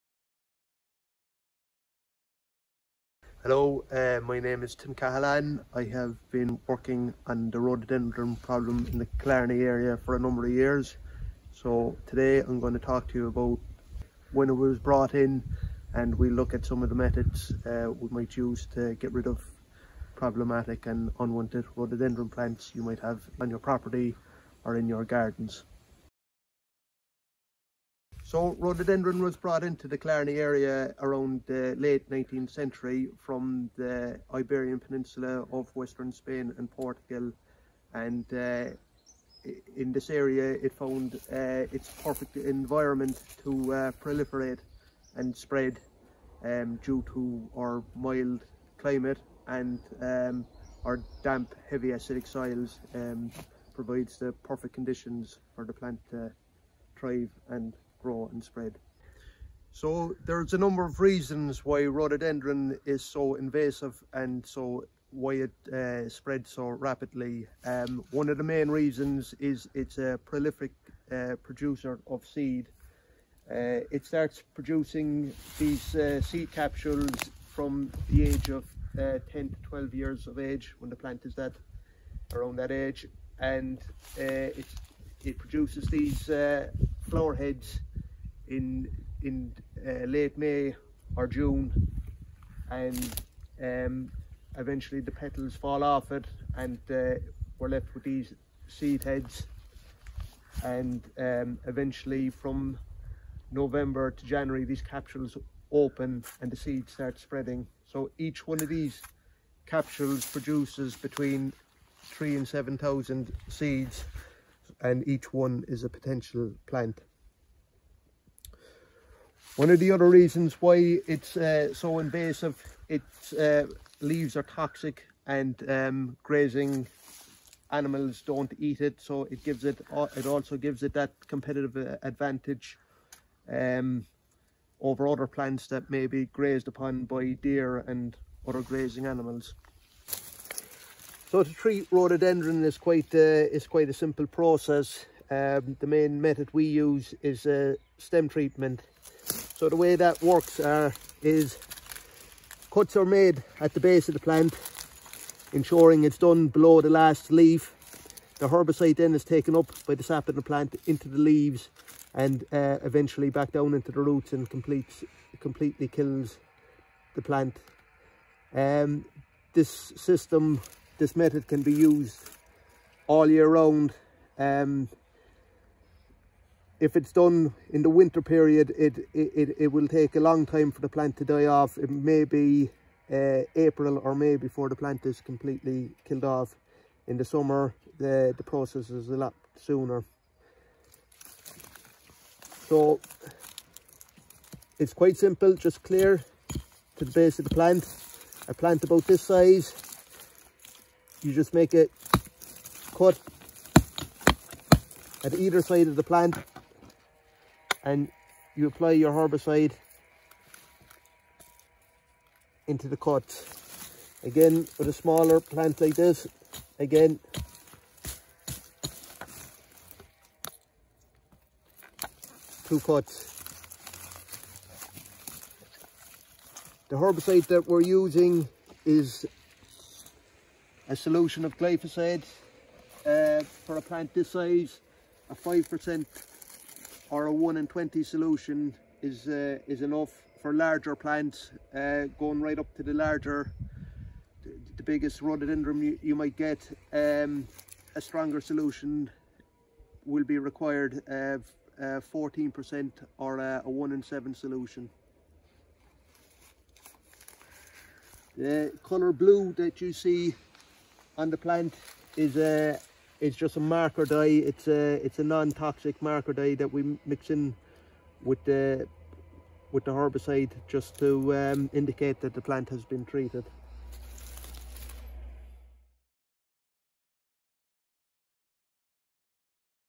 Treatment of Rhododendron - Hatchet method
An informative video about the invasive species Rhododendron ponticum and how to effectively treat it to prevent further spread in unwanted areas.